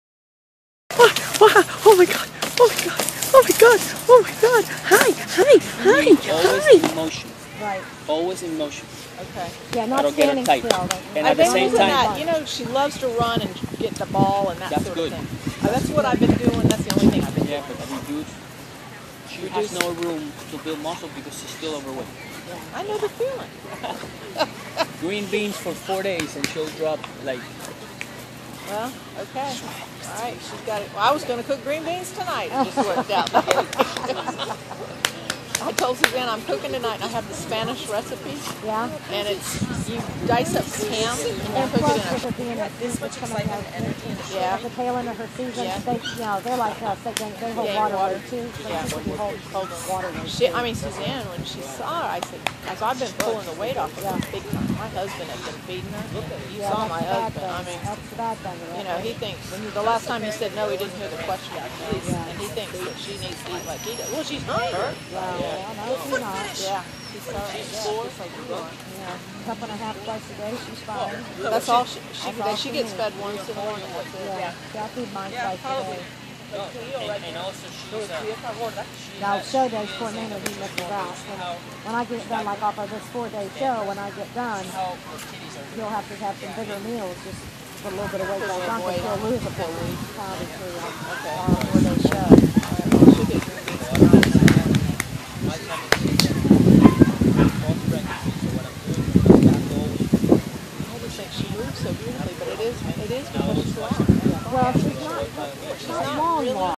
Mira's Reaction to Me Showing Up in Ocala - 11.14.2013
This is how my little porkchop reacted when she saw me after showing in Ocala.